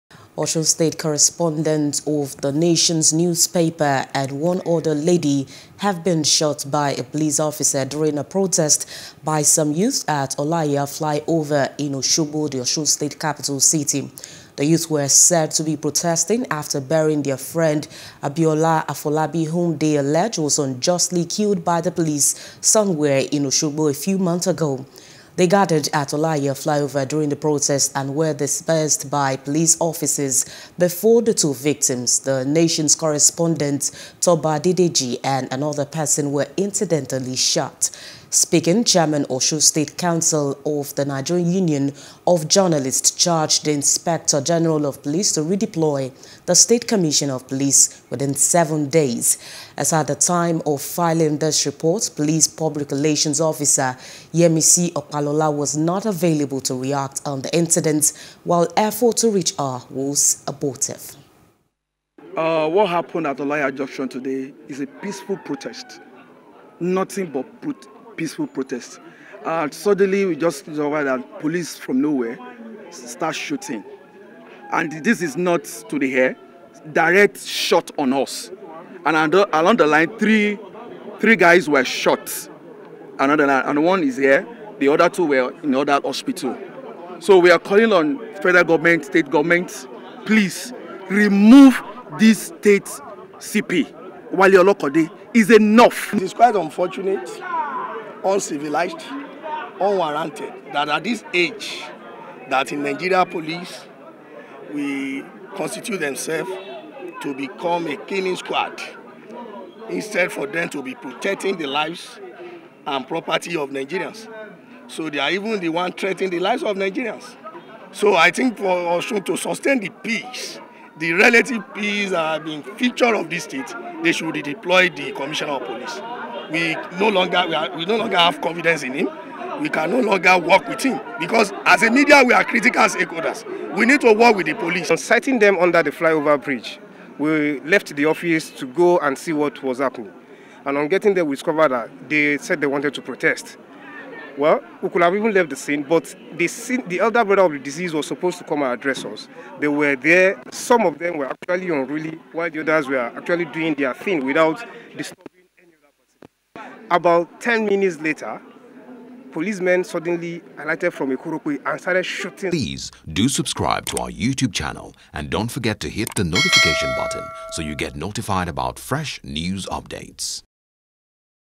Protest: Police Shoot Journalist In Osun | NEWS
Osun state correspondent of The Nation's newspaper and one other lady have been shot by a police officer during a protest by some youths at olaiya flyover in Osogbo, the Osun state capital city.